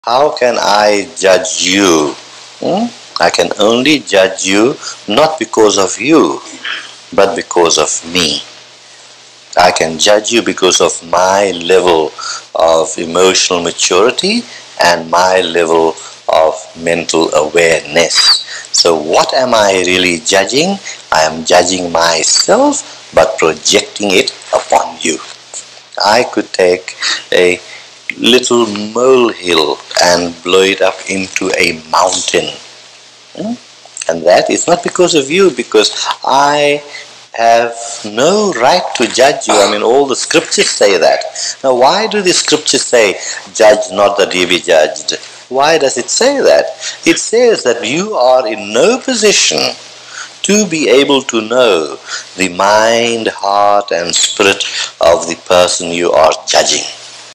How can I judge you?
How can I judge you? I can only judge you, not because of you, but because of me. I can judge you because of my level of emotional maturity, and my level of mental awareness. So what am I really judging? I am judging myself, but projecting it upon you. I could take a little molehill and blow it up into a mountain. And that... it's not because of you, because I have no right to judge you. I mean, all the scripture say that. Now, why do the scriptures say, "Judge not that ye be judged?" Why does it say that? It says that you are in no position to be able to know the mind, heart, and spirit of the person you are judging.